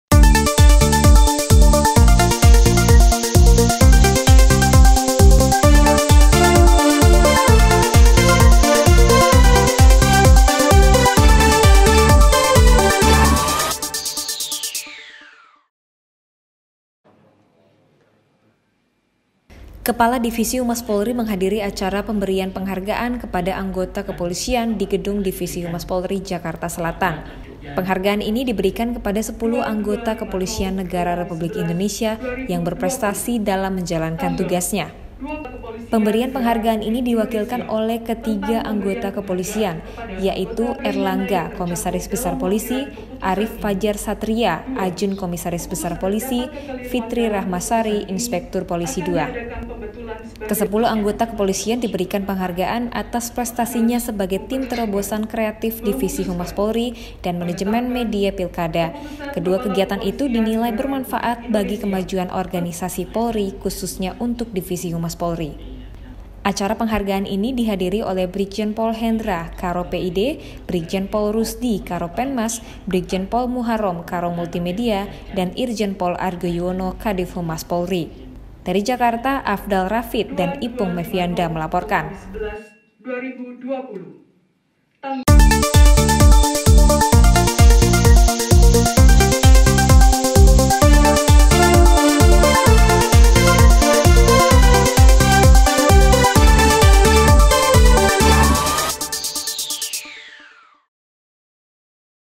PEMBERIAN PENGHARGAAN KEPADA ANGGOTA DIVISI HUMAS POLRI YANG BERPRESTASI
Kadiv Humas Polri menghadiri acara pemberiaan penghargaan, kepada anggota kepolisian, di gedung Divisi Humas Polri Jakarta Selatan. Penghargaan ini diberikan kepada sepuluh anggota kepolisian negara republik indonesia yang berprestasi dalam menjalankan tugasnya.

Pemberian penghargaan ini diwakilkan oleh ketiga anggota kepolisian yaitu Erlangga Komisaris Besar Polisi, Arief Fajar Satria, Ajun Komisaris Besar Polisi, Fitri Rahmasari Inspektur Polisi II.

Kesepuluh anggota kepolisian diberikan penghargaan, atas prestasinya sebagai tim terobosan kreatif divisi humas polri, dan manajemen media pilkada, kedua kegiatan itu dinilai bermanfaat bagi kemajuan organisasi polri khususnya untuk divisi humas polri.

Acara penghargaan ini dihadiri oleh Brigjen Pol Hendra Karo PID, Brigjen Pol Rusdi Karo Penmas, Brigjen Pol Muharrom Karo Multimedia dan Irjen Pol Argo Yuwono Kadiv Humas Polri.
Tim liputan : Ipung dan Afdal
Melaporkan.